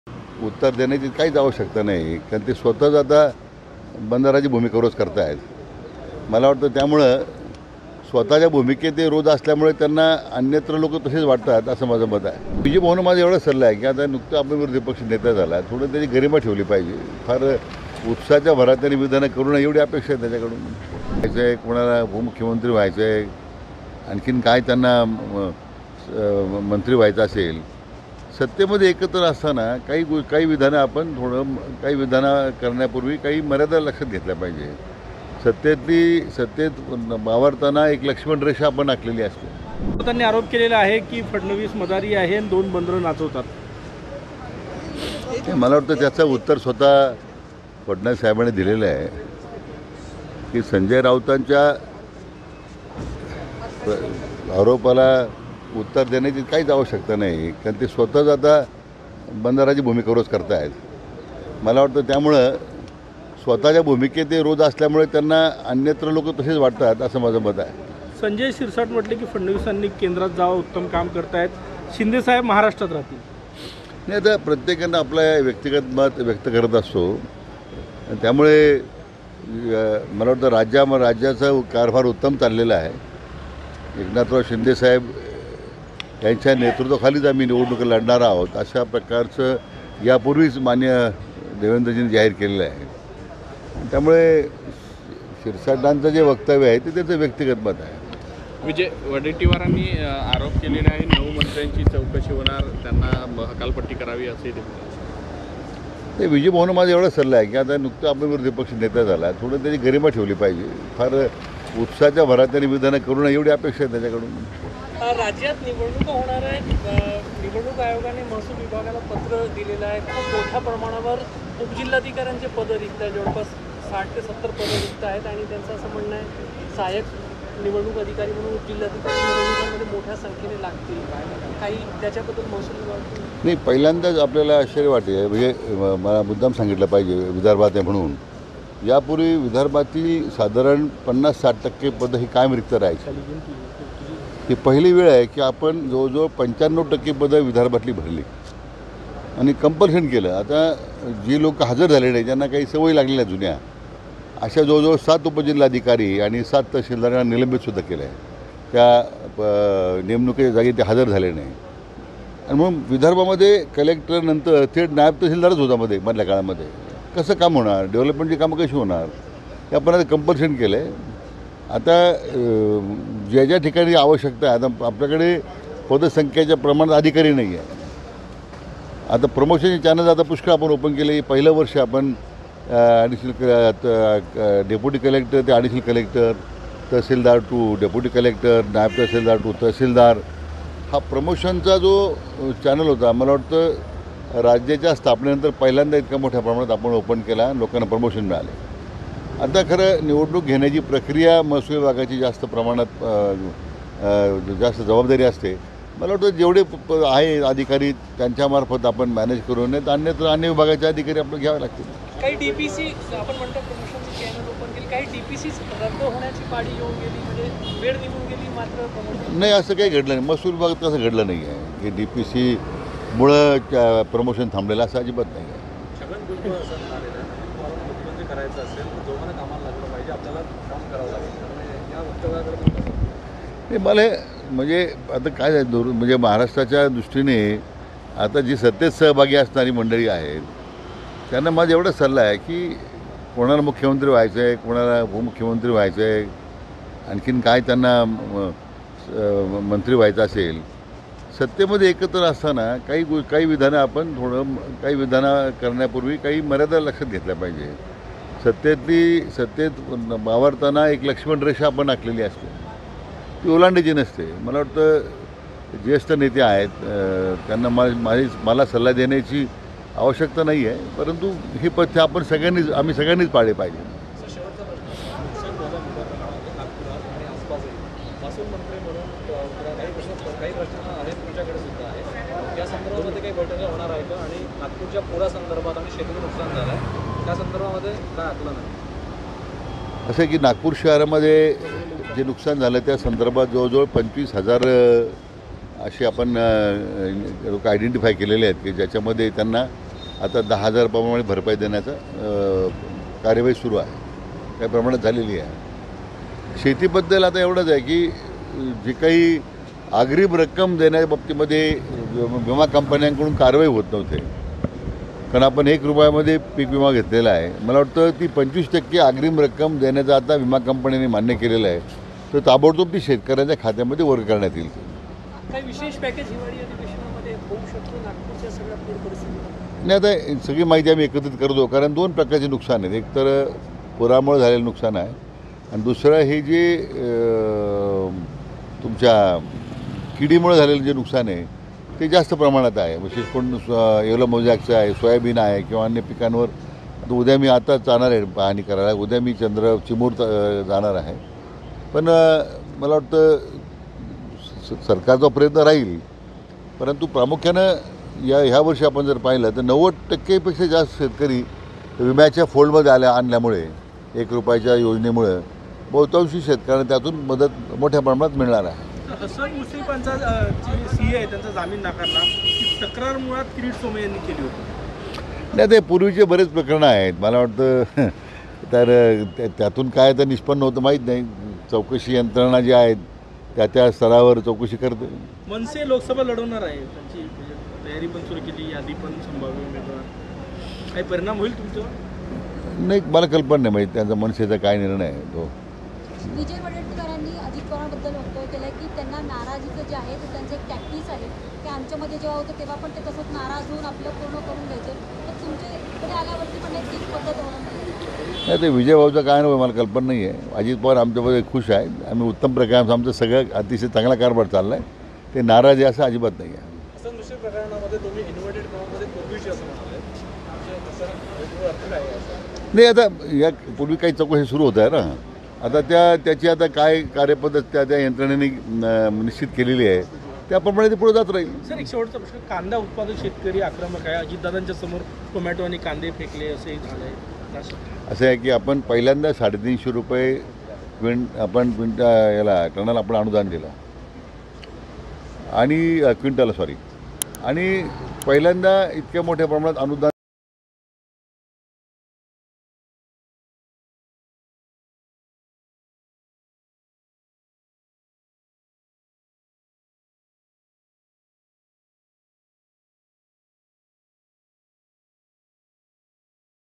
Radhakrishna Vikhe Patil | सत्तेत सहभागी असणाऱ्यांनी मर्यादा लक्षात घ्याव्या... | LetsUpp Marathi
महसूल मंत्री राधाकृष्ण विखे पाटील यांनी मंत्री छगन भुजबळ यांना जाहीरपणे इशारा दिला आहे. सत्तेत सहभागी असणाऱ्यांनी आपल्या मर्यादा लक्षात घ्याव्या असंही मंत्री विखे म्हणाले.